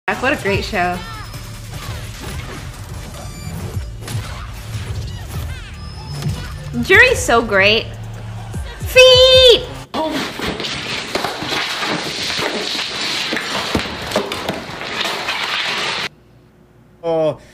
Susu- Reacting to Juri Feet
I think Susu's impressed by the new trailer. At it looks like she's not the only one.